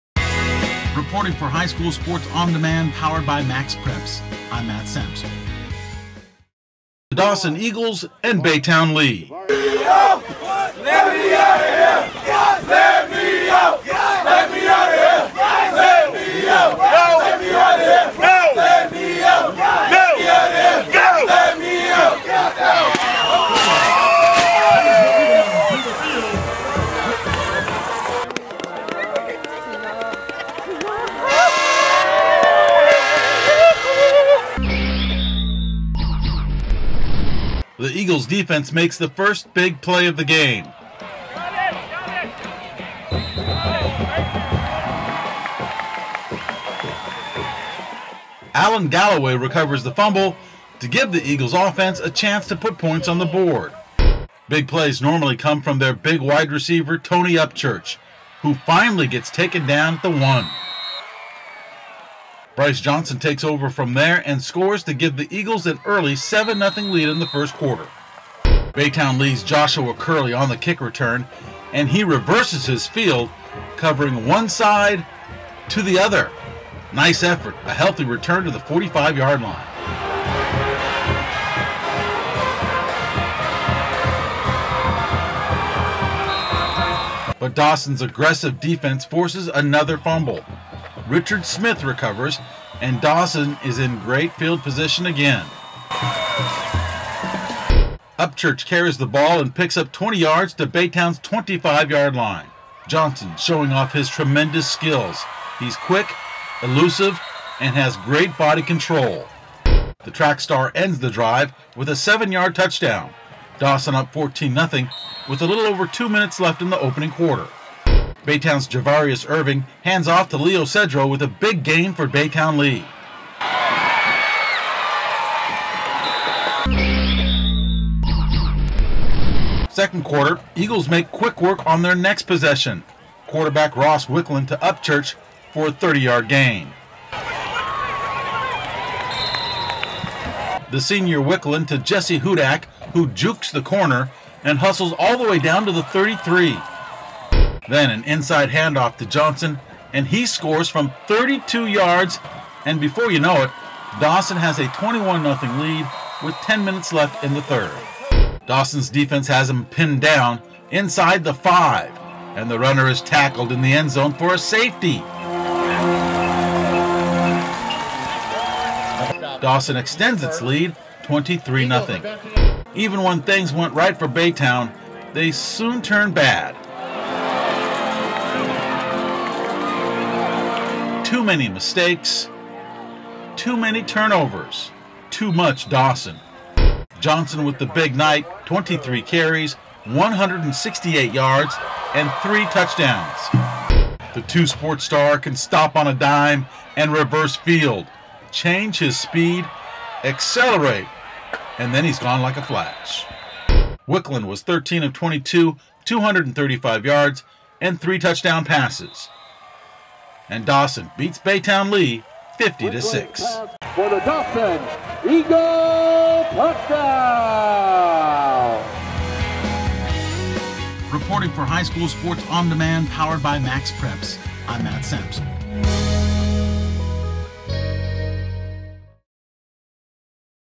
Football Lee vs dawson October, 18, 2013
HISD Lee high school takes on the Dawson Eagles of Pearland. REporter, Matt Sampsell